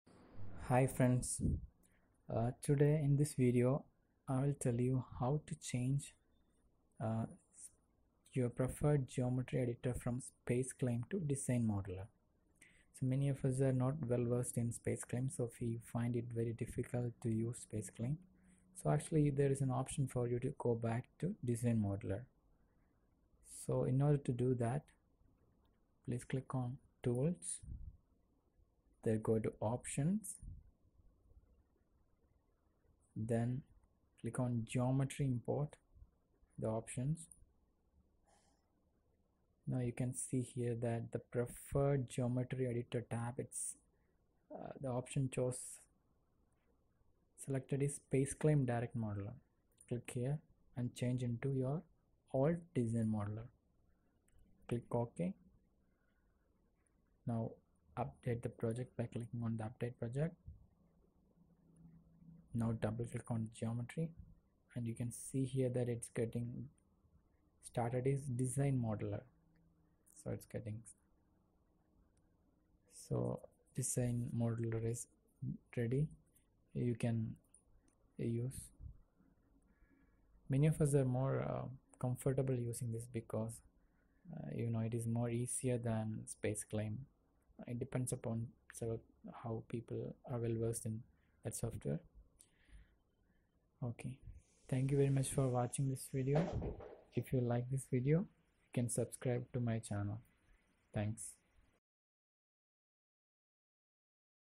How to use Ansys Design Modeler instead of SpaceClaim in Ansys 17.2
Space claim Geometry editor opens whenever you opens Ansys 17.2 Geometry. Follow steps in this video to edit your geometry in old Design Modeler.